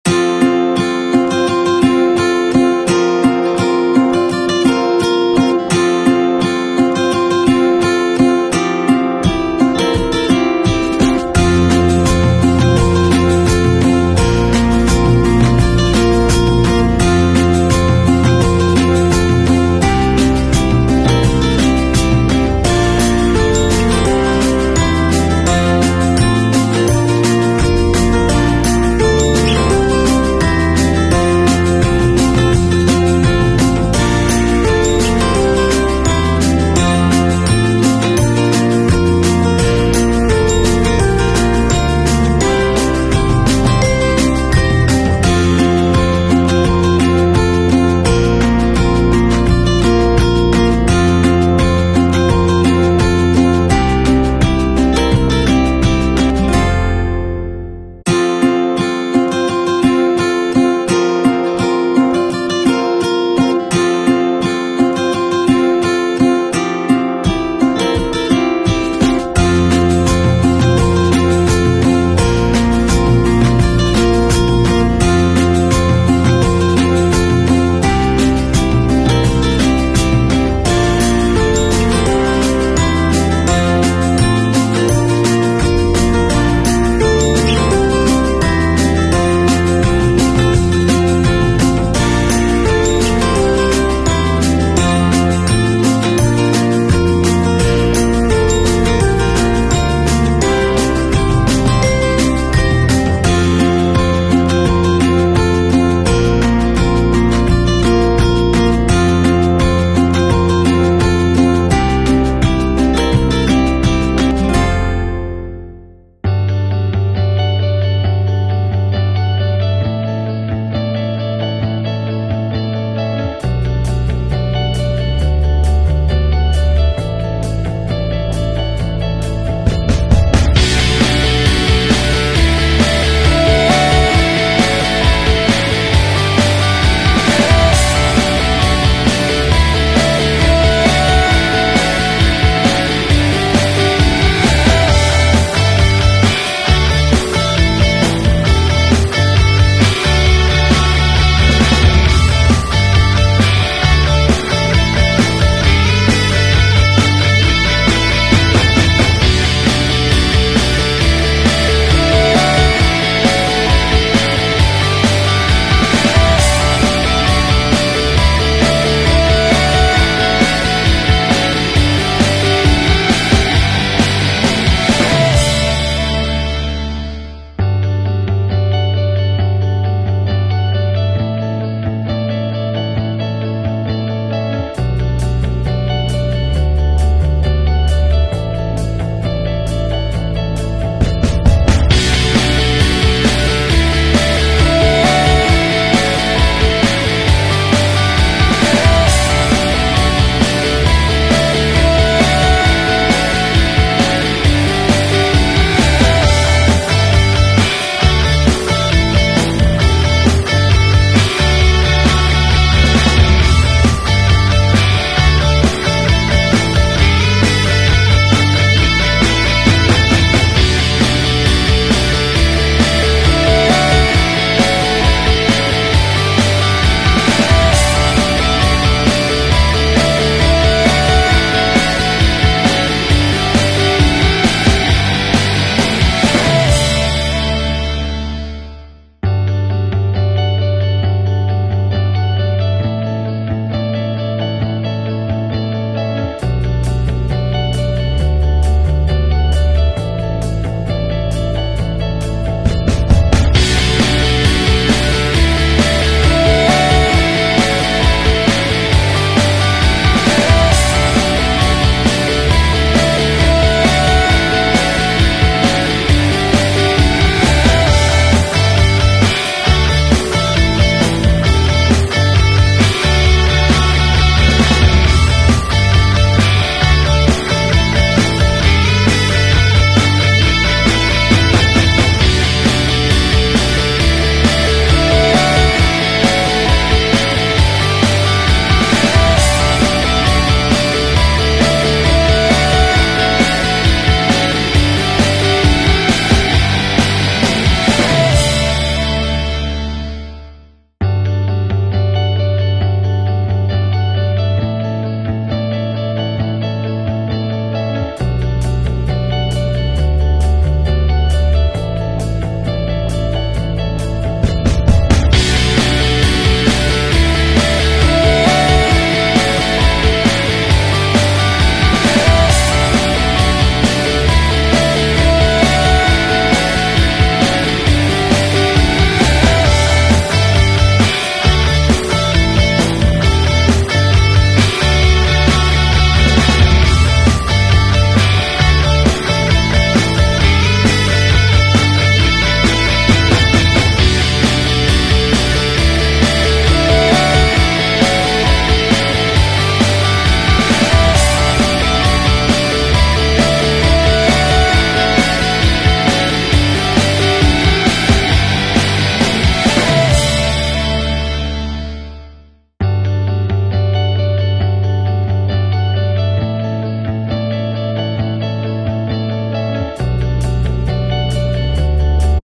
dji mini 2 Stoke City FC training ground footage
flying over stoke city training ground